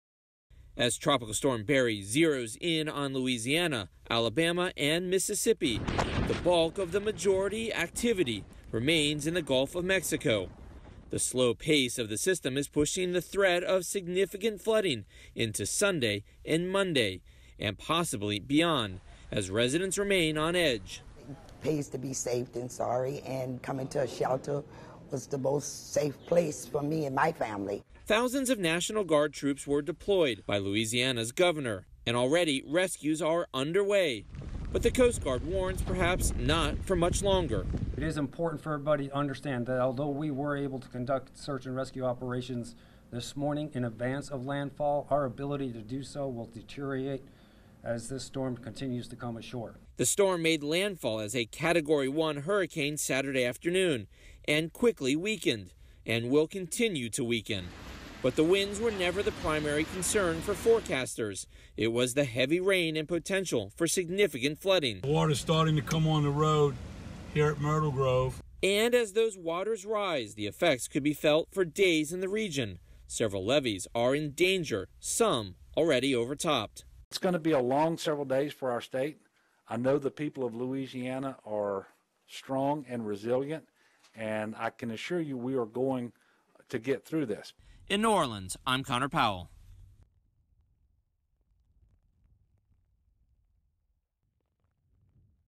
Weakened Barry rolls into Louisiana, drenches Gulf Coast
Barry rolled into the Louisiana coast Saturday, flooding highways, forcing people to scramble to rooftops and dumping heavy rain that officials had feared could test the levees and pumps that were bolstered after Hurricane Katrina devastated New Orleans in 2005.

After briefly becoming a Category 1 hurricane, the system weakened to a tropical storm as it made landfall near Intracoastal City, about 160 miles (257 kilometers) west of New Orleans, the National Hurricane Center said.

By early evening, New Orleans had been spared the worst effects, receiving only light showers and gusty winds. A National Weather Service forecaster said the city may escape with only 2 to 4 inches (5 to 10 centimeters) of rain. But officials warned that Barry could still cause disastrous flooding across a wide stretch of the Gulf Coast and drop up to 20 inches (50 centimeters) through Sunday across other parts of Louisiana.

"This is just the beginning," Louisiana Gov. John Bel Edwards said. "It's going to be a long several days for our state."